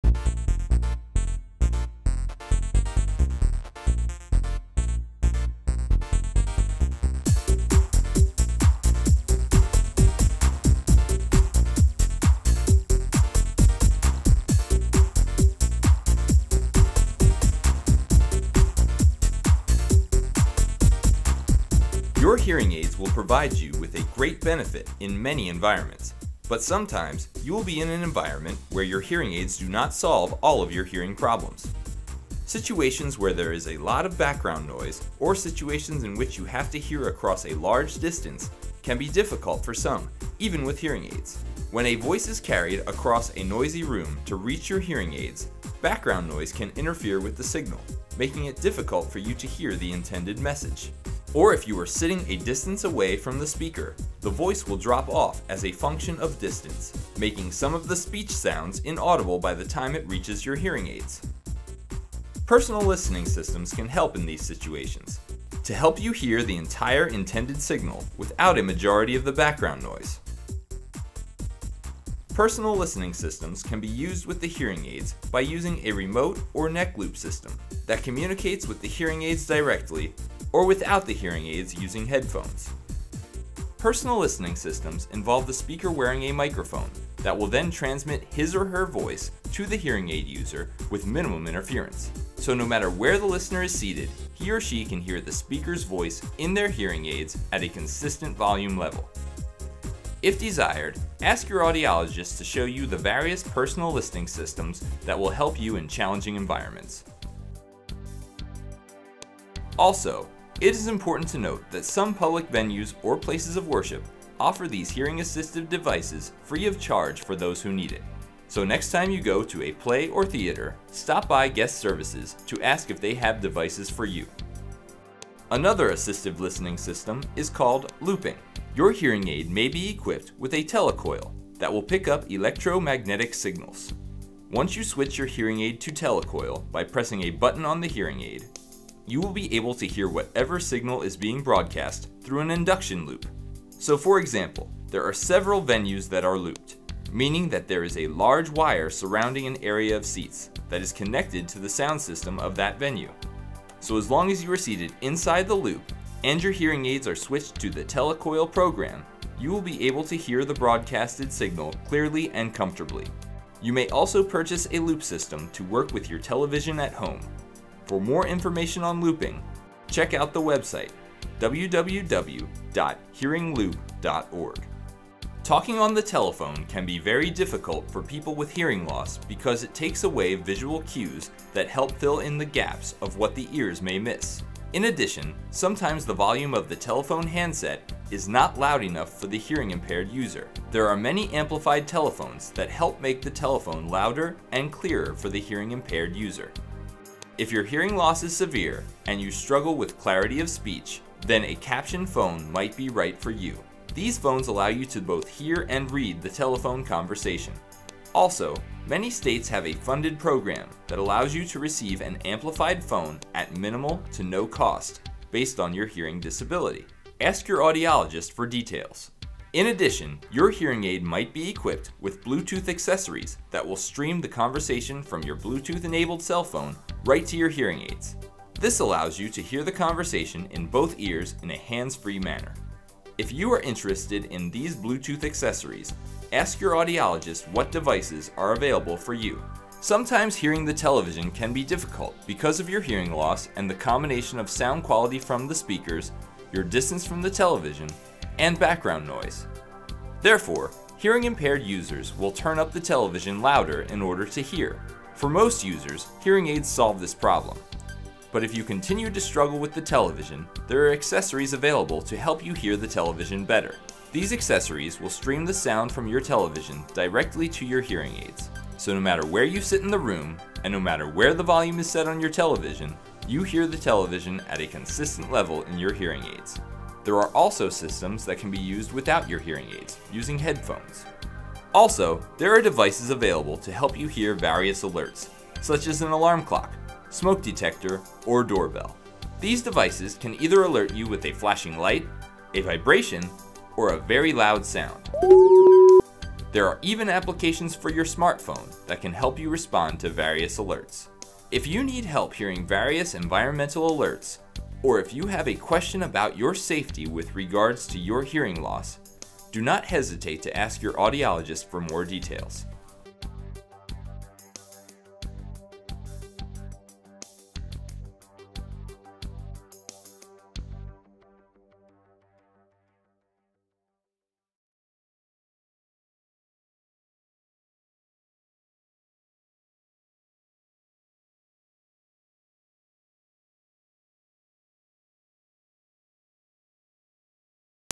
Hearing Assistive Technology
This video provides an overview of hearing assistive technology (HAT) available for those with hearing loss. These HATs can be used independently or in conjunction with hearing aids or cochlear implants. If you feel that you or a loved one would benefit from any of these devices, please contact your Audiologist.